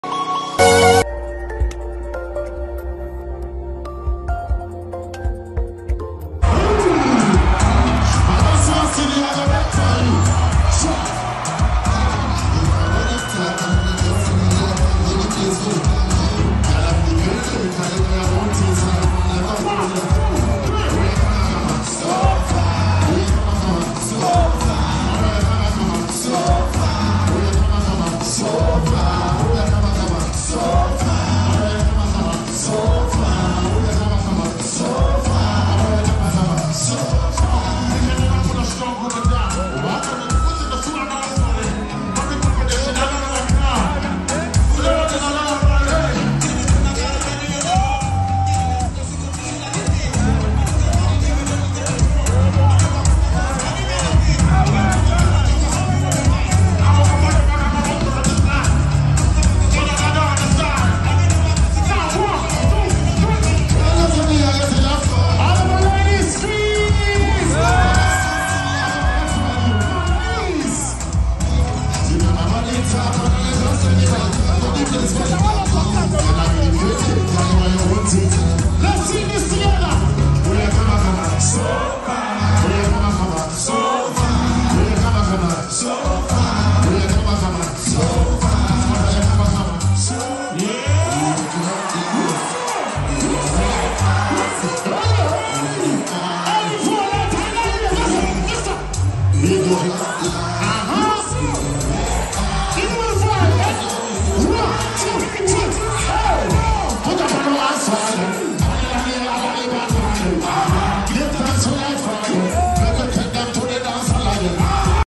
Stonebwoy performs with Sarkodie at RAPPERHOLIC X Concert .
SARKODIE and STONEBWOY SETTLE THEIR BEEF AS THEY PERFORM TOGETHER AT RAPPERHOLIC X CONCERT